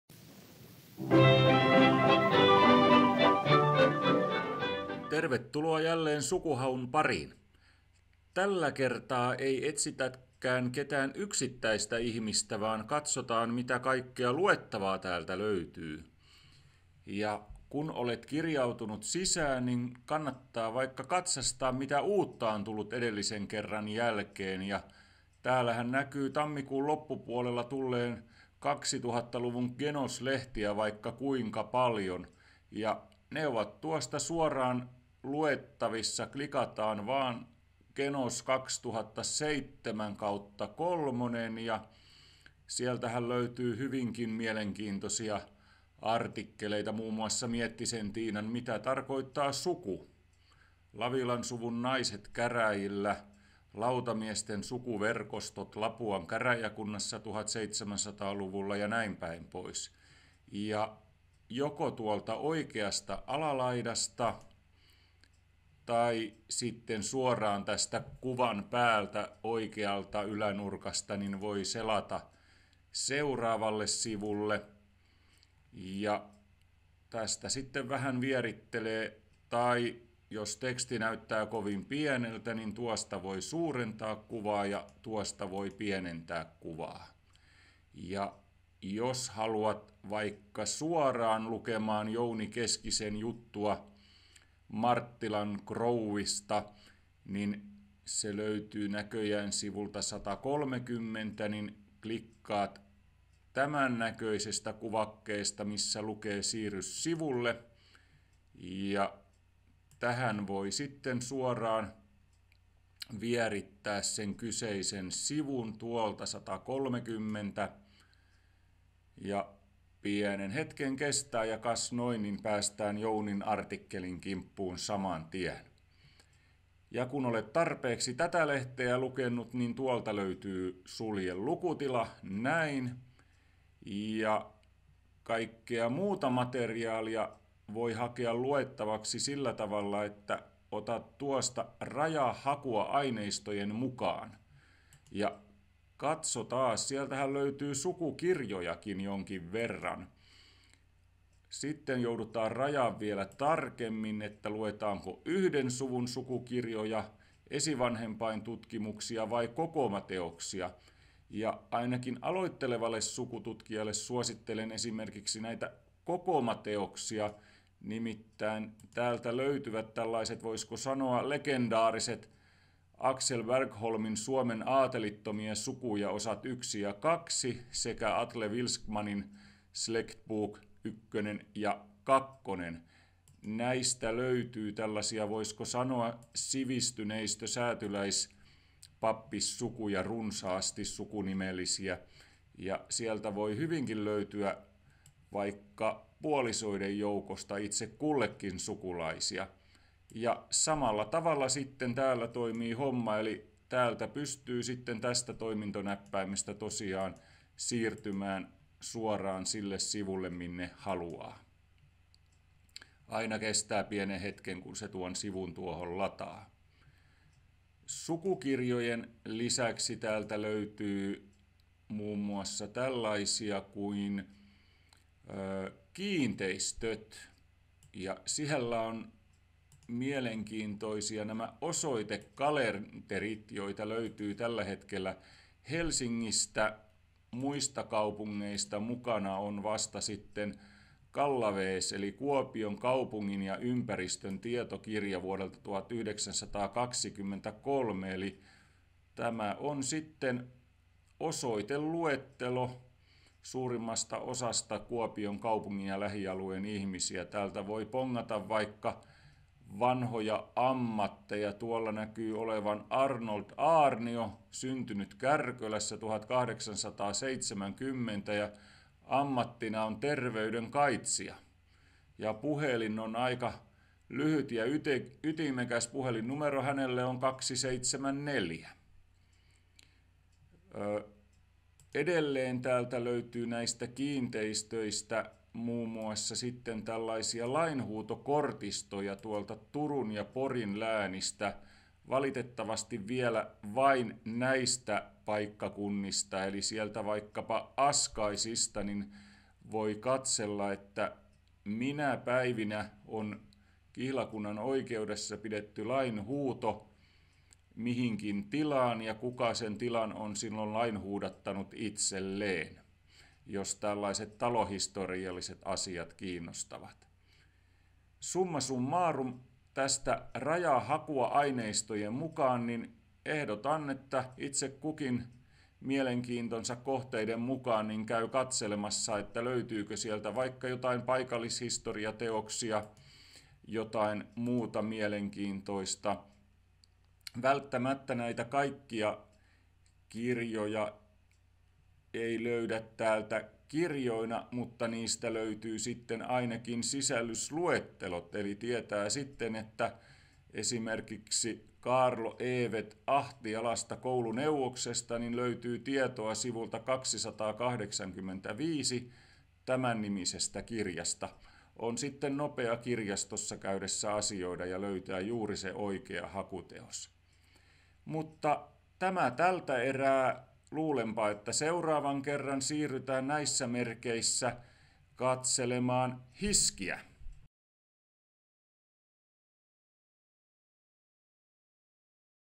Miten käytän SukuHakua? Osa 2
Suomen Sukututkimusseuran uuden opastevideosarjan toinen osa, jossa perehdytään Sukuhaun ominaisuuksiin.